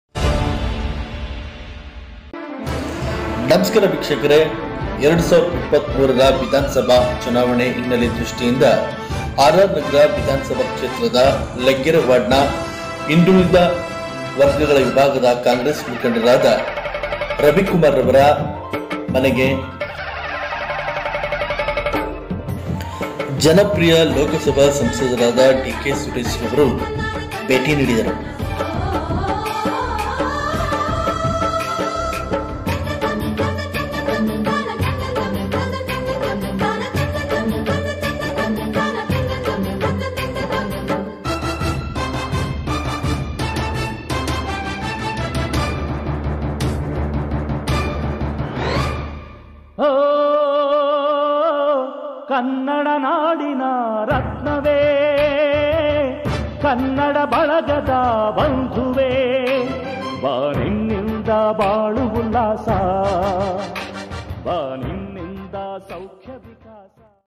ಲಗ್ಗೆರೆ ವಾರ್ಡ್ ಕಾಂಗ್ರೆಸ್ ಮುಖಂಡರಾದ ರವಿಕುಮಾರ್ ರವರ ಮನೆಗೆ ಜನಪ್ರಿಯ ಲೋಕಸಭಾ ಸಂಸದರಾದ ಡಿಕೆ ಸುರೇಶ್
ಭೇಟಿ ನೀಡಿದರು